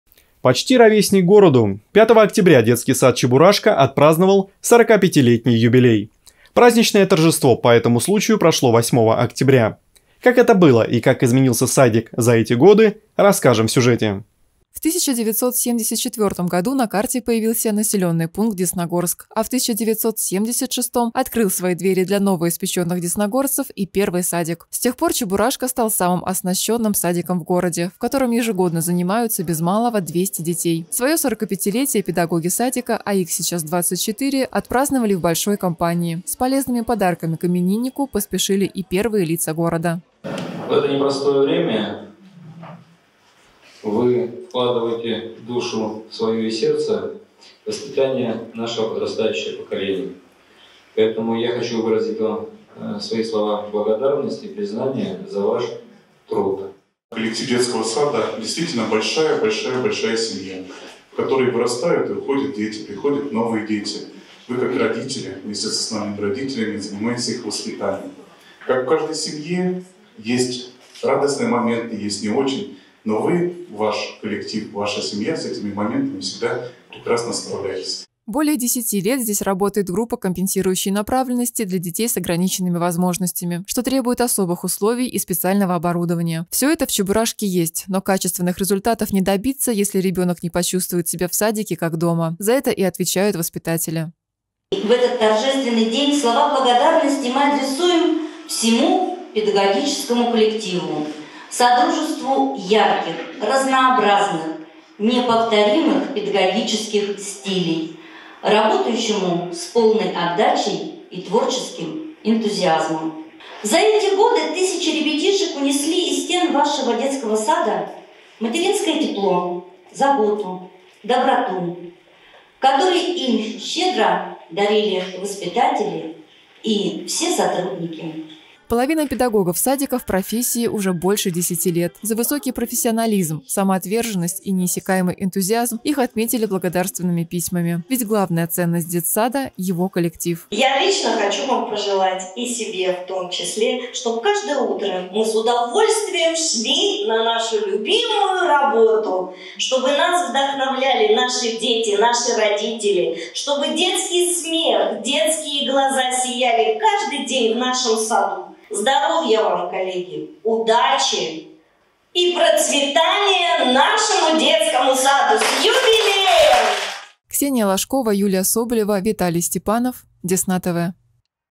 Десна-ТВ: «Чебурашке» - 45: первый садик Десногорска отметил юбилей
Почти ровесник городу. 5 октября детский сад «Чебурашка» отпраздновал 45-летний юбилей. Праздничное торжество по этому случаю прошло 8 октября. Как это было и как изменился садик за эти годы, смотрите в сюжете.